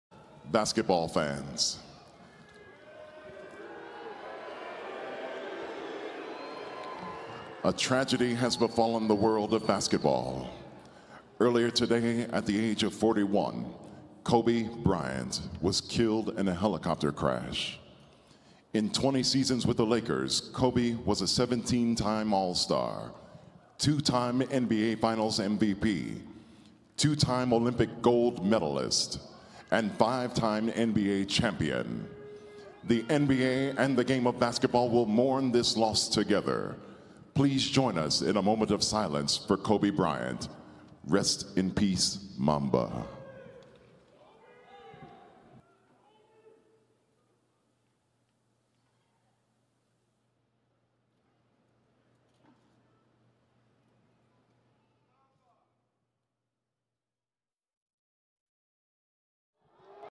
Rockets and Nuggets Hold Moment of Silence to Mourn Kobe Bryant’s Death 🙏
Fastest NBA full game highlights